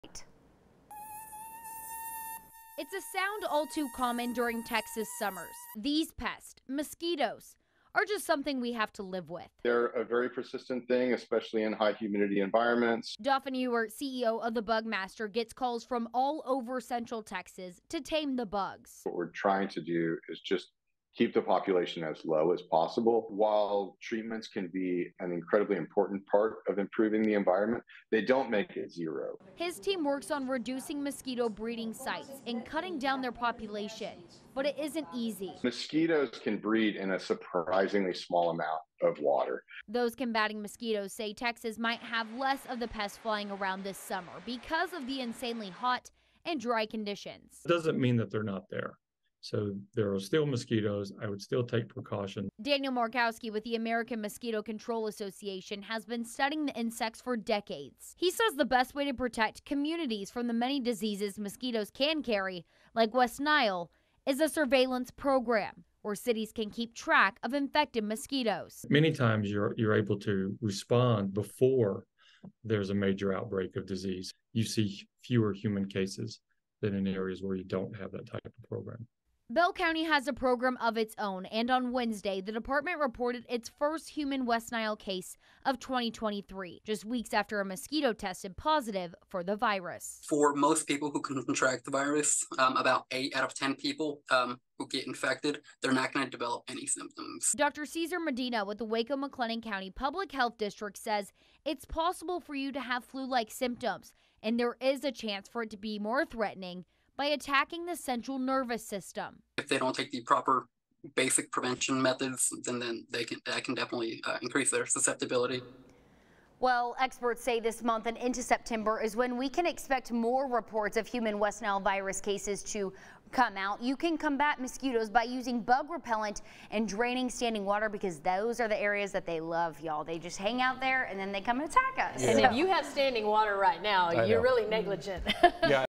First person diagnosed with West Nile Virus in Bell County
The Bell County Public Health District said humans are generally at low risk of serious infection, but steps can be taken to minimize chances of infection.